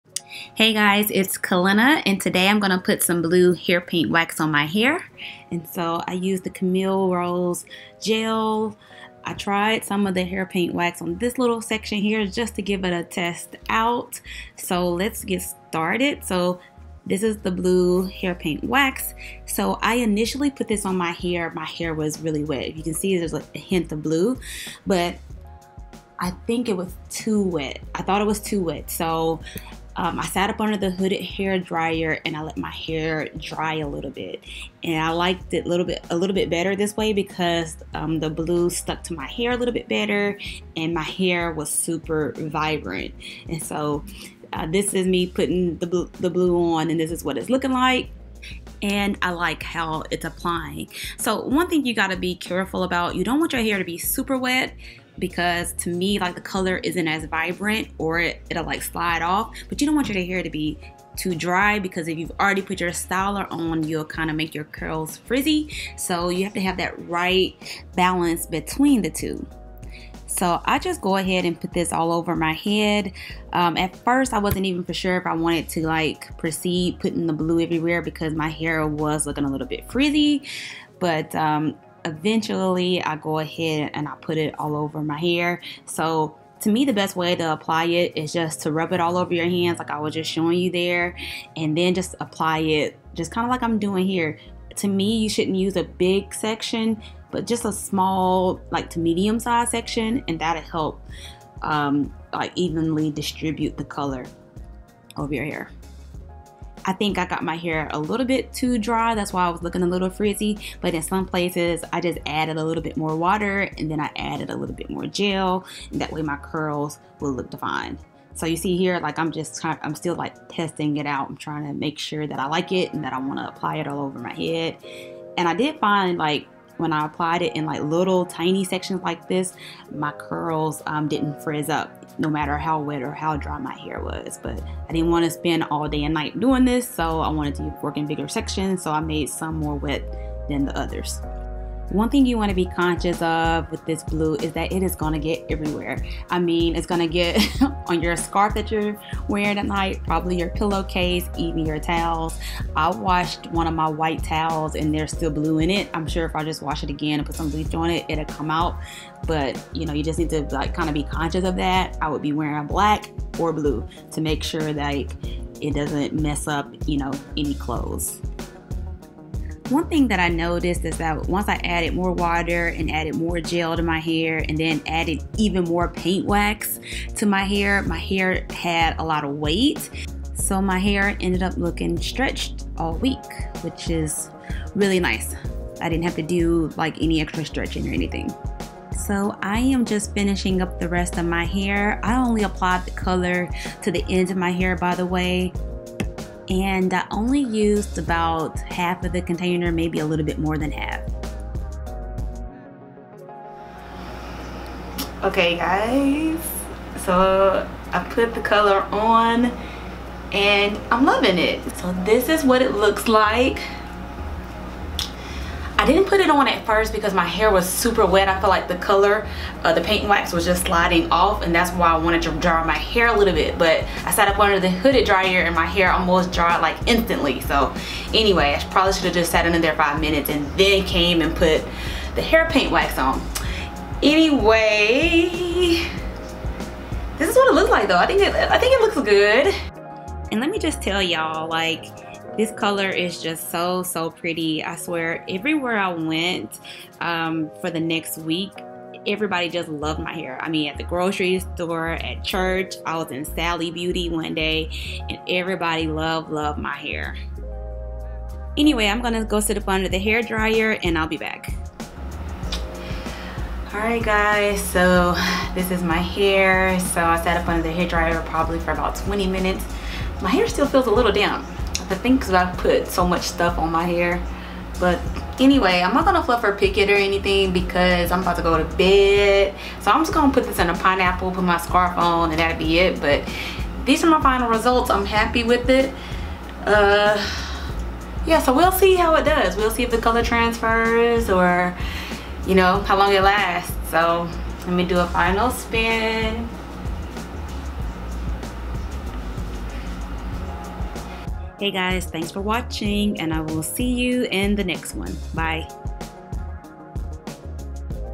I Tried Blue Hair Paint Wax on My Curly Natural Hair Tutorial!!!! BOMB Results 😍
Hey guys! I tried blue hair paint wax on my natural hair! Let me know if you would give it a try!

Products Used: Blue Hair Paint Wax

Codes:
Curlmix: curlycalynna
Kaleidoscope: Calynna20